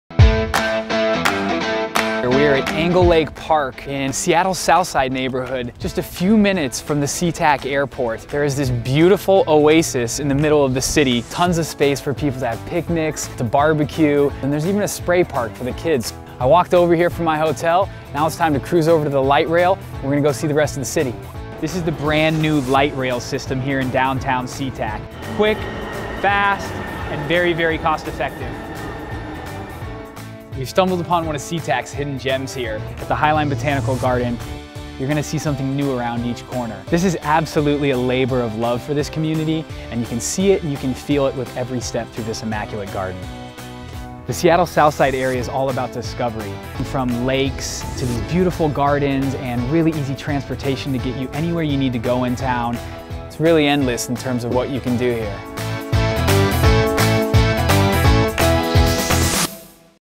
Explore the Seattle Southside Neighborhood: SeaTac
SeaTac is one of the three cities that makes Seattle Southside a great place to stay, play, eat and explore. Learn more about this exciting destination from a travel expert.